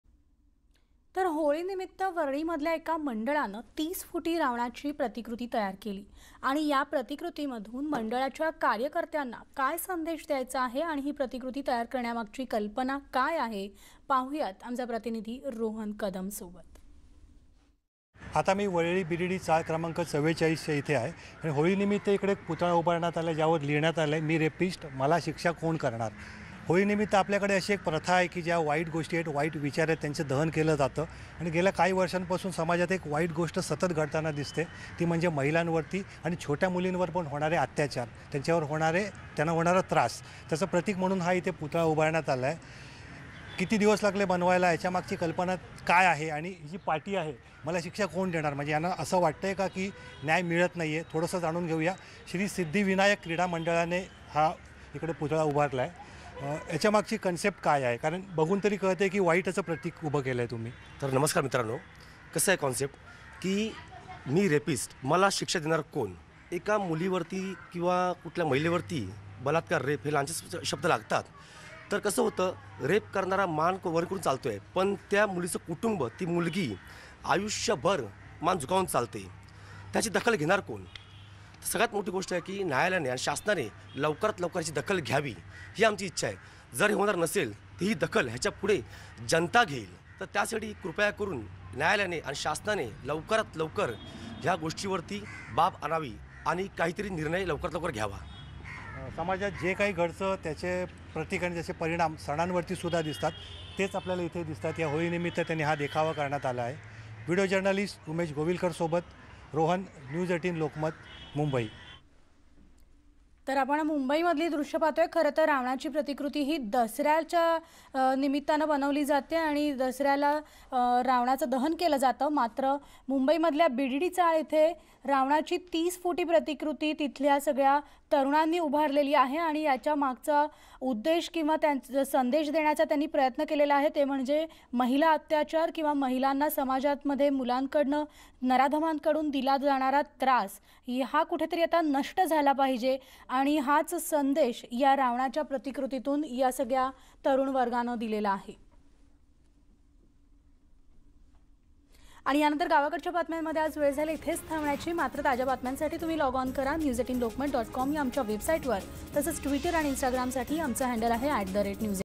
महिला अत्यचारांच्याविरोधात तरुणाईचा आवाज, होळीनिमित्त करणार वाईट प्रवृत्तींचं दहन
महिला अत्यचारांच्याविरोधात तरुणाईचा आवाज, होळीनिमित्त करणार वाईट प्रवृत्तींचं दहन   पहा होळीनिमित्त स्पेशल रिपोर्ट थेट मुंबईतून।

/News18Lokmat